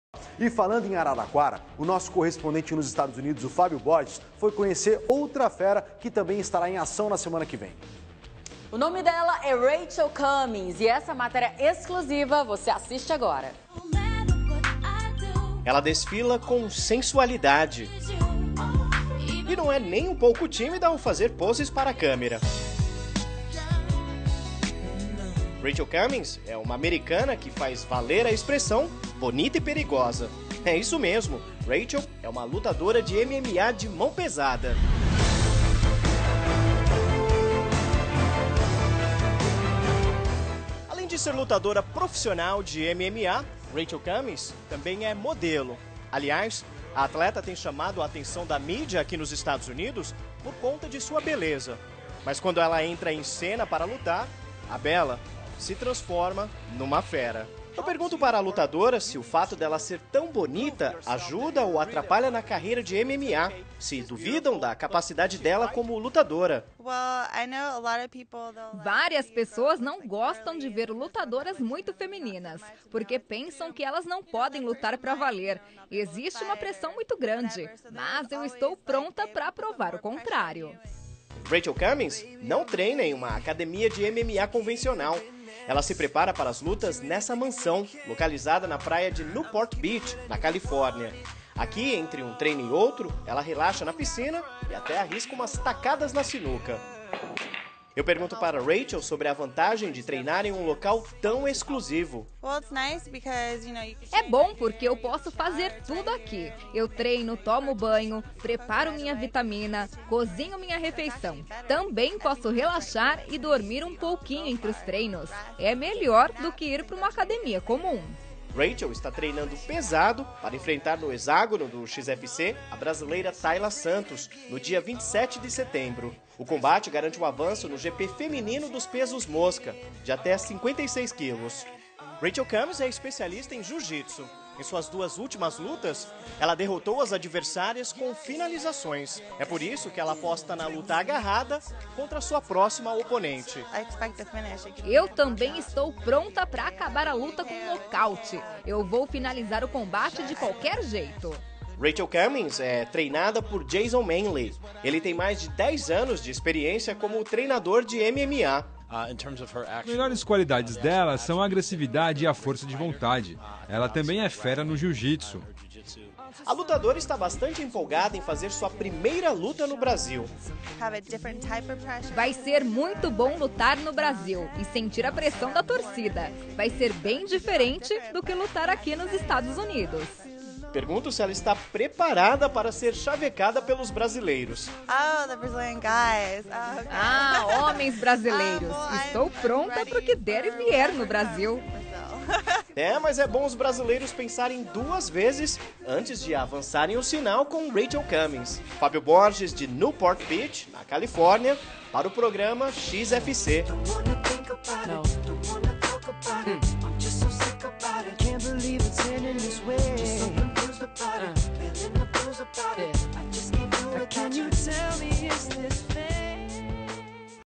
XFC na RedeTV!: Exclusivo! Rachel Cummins: 'estou pronta para o que der e vier no Brasil'
O XFC entrevistou com exclusividade a lutadora Rachel Cummins, que enfrenta Taila Santos no XFCi 6